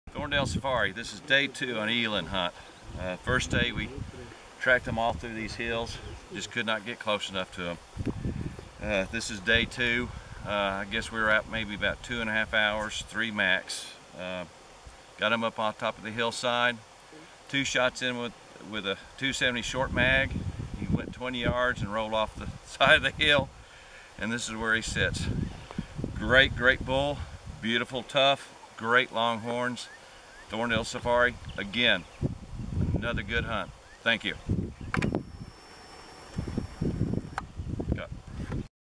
Thorndale Safari 2015“Safari After The Hunt Unedited" Trophy Eland
Thorndale Safari 2015 Trophy Eland, 2378 lbs shot with a .270 WSM,
 150gr Barnes TSX bullet Outside of Kirkwood, South Africa.